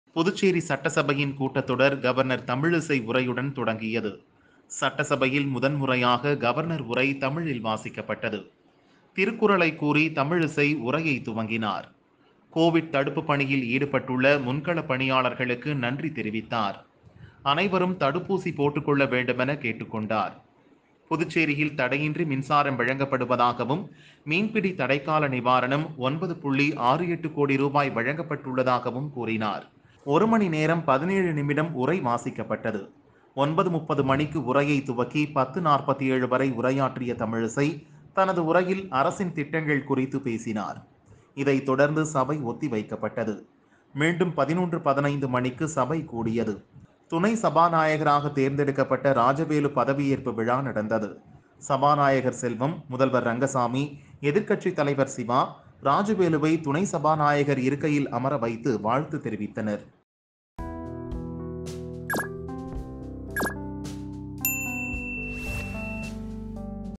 திருக்குறளுடன் தொடங்கிய புதுவை சட்டசபை கூட்டத்தொடர்
திருக்குறளுடன்
தொடங்கிய
புதுவை சட்டசபை